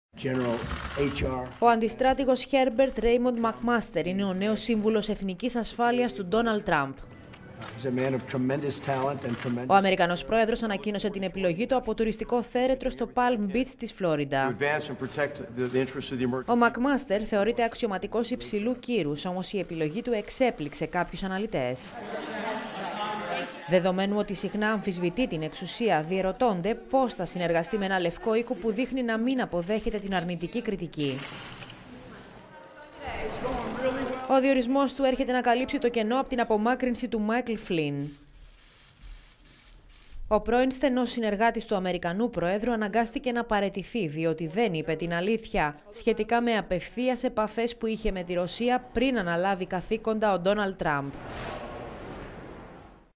Ποιος είναι ο νέος σύμβουλος Εθνικής Ασφάλειας του Ντόναλντ Τραμπ
Ο αντιστράτηγος Χέρμπερτ Ρέιμοντ Μακμάστερ είναι ο νέος σύμβουλος Εθνικής Ασφάλειας του Ντόναλντ Τραμπ.

Ο Αμερικανός πρόεδρος ανακοίνωσε την επιλογή του από τουριστικό θέρετρο στο Παλμ Μπιτς της Φλόριντα.

Congratulations to our new National Security Advisor, General H.R. McMaster! President Trump (@POTUS) February 20, 2017

Ο Μακμάστερ θεωρείται αξιωματικός υψηλού κύρους, όμως η επιλογή του εξέπληξε κάποιους αναλυτές.